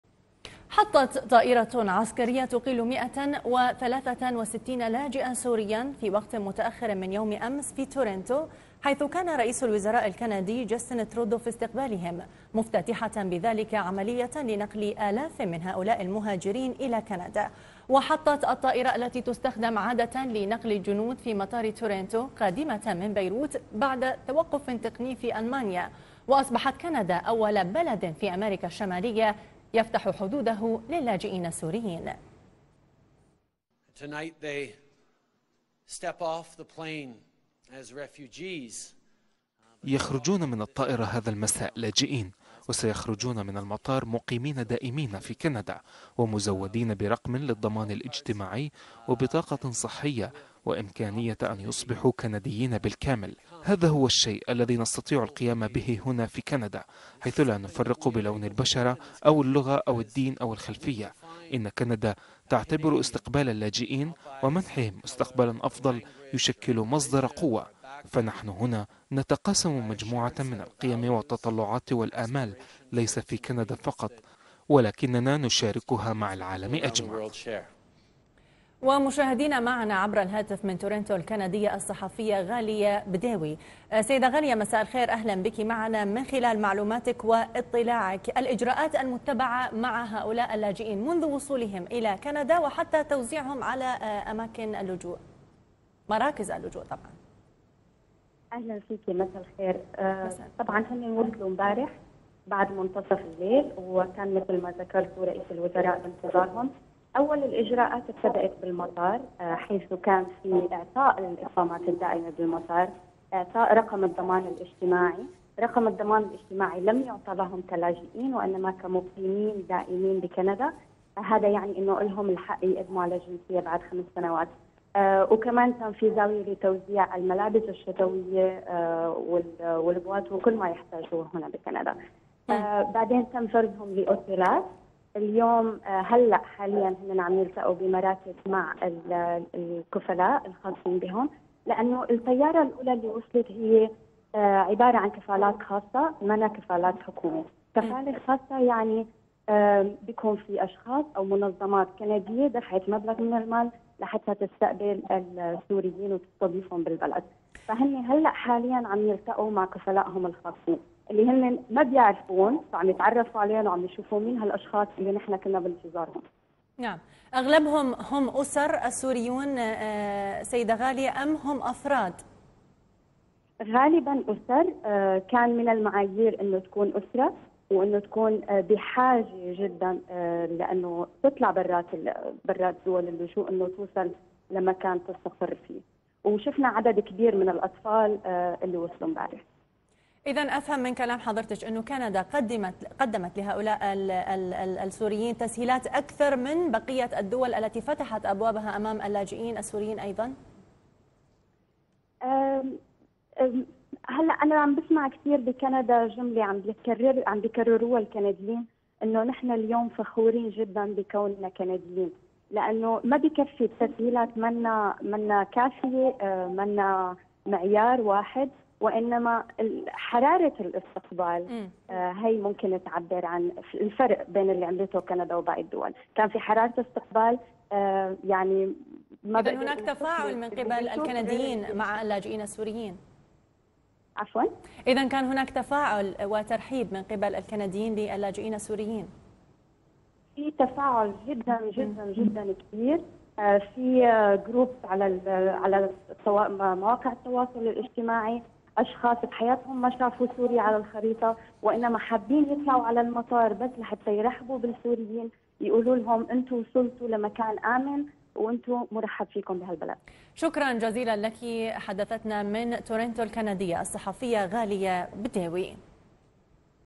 وصول اول طائرة للاجئين سوريين الى مدينة تورونتو الكندية
شاهد آيضا:
قيادي بعثي يفضح تاريخ آل الأسد!؟
حطت طائرة عسكرية تقل 163 لاجئا سوريا في وقت متأخر من الخميس في تورونتو حيث كان رئيس الوزراء الكندي جاستن ترودو في استقبالهم، مدشنة بذلك عملية لنقل آلاف من هؤلاء المهاجرين الى كندا، وحطت الطائرة التي تستخدم عادة لنقل الجنود في المطار قادمة من بيروت بعد توقف تقني في المانيا.

وقال ترودو ان كندا تعتبر ان استقبال هؤلاء اللاجئين ومنحهم مستقبلا افضل لهم ولابنائهم برحابة صدر يشكل "مصدر قوة".

وقال ترودو قبل ان يرحب بالعائلات ومعظمها سيدات مع ابنائهن "سنتذكر جميعا هذا اليوم".

واصبحت كندا في عهد الحكومة الليبرالية الجديدة التي يقودها ترودو اول بلد في اميركا الشمالية يفتح حدوده للاجئين السوريين.